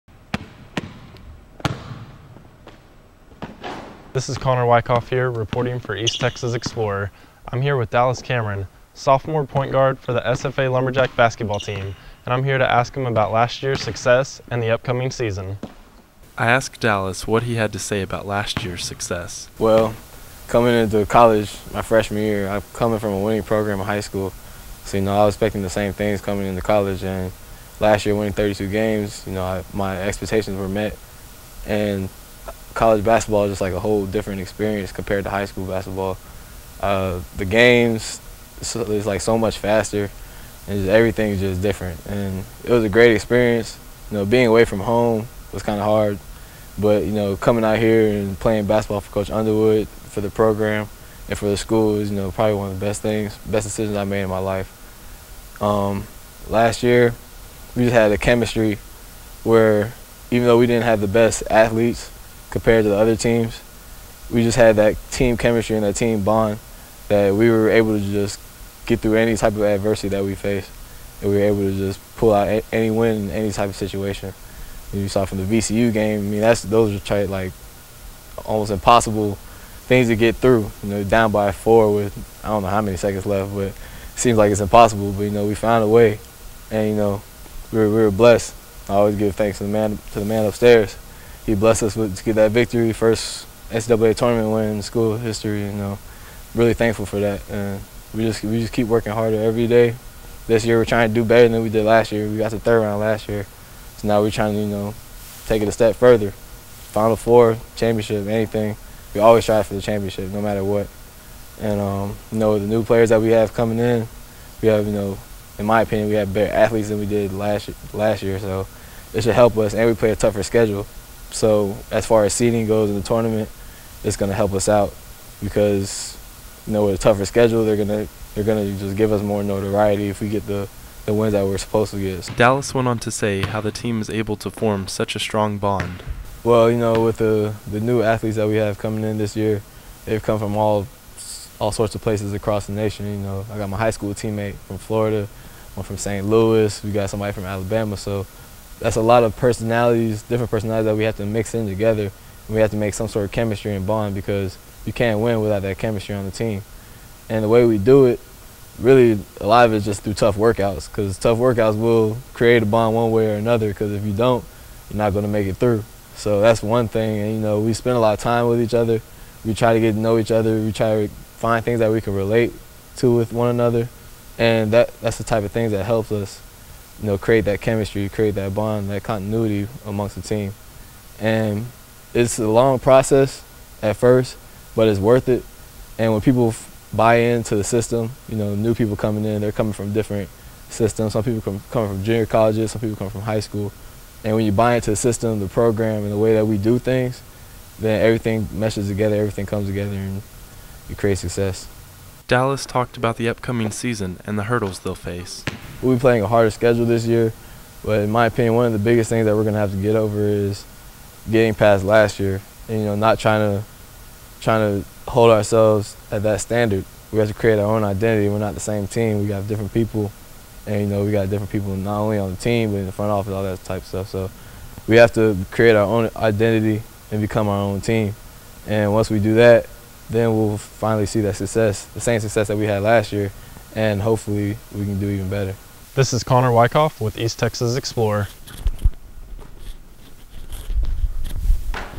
SFA Basketball: Dallas Cameron
2014 interview with Stephen F. Austin PG Dallas Cameron.